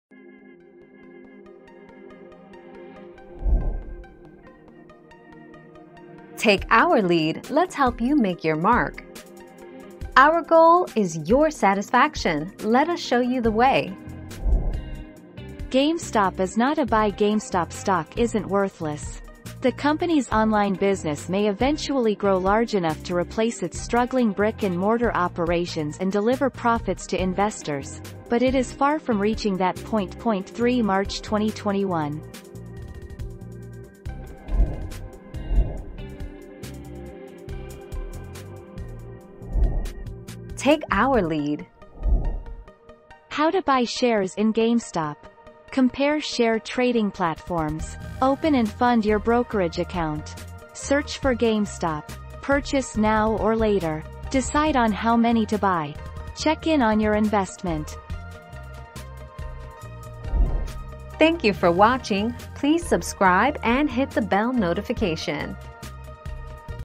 Is GameStop stock worth buying?
00:00 - Is GameStop stock worth buying?
00:36 - How do I invest in GameStop stock?

Laura S. Harris (2021, April 20.) Is GameStop stock worth buying?
    AskAbout.video/articles/Is-GameStop-stock-worth-buying-238684